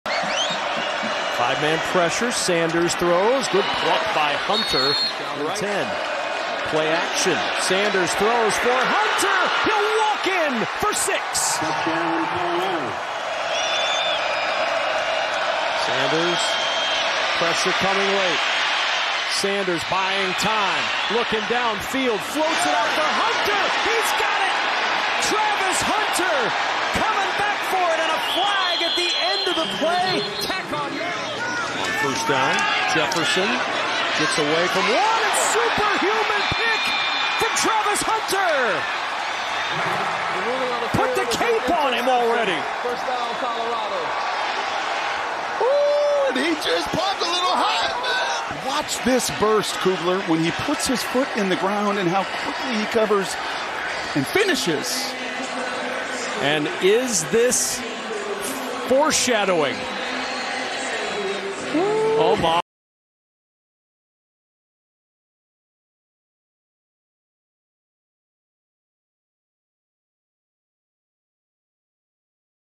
Travis Hunter Hits Heisman Pose in 1 INT, 1 TD Performance vs. UCF
Travis Hunter highlights vs. UCF as the Colorado two-way star goes for 9 catches, 89 yards, a touchdown, and an incredible diving interception that he had to hit the Heisman pose for. 🔥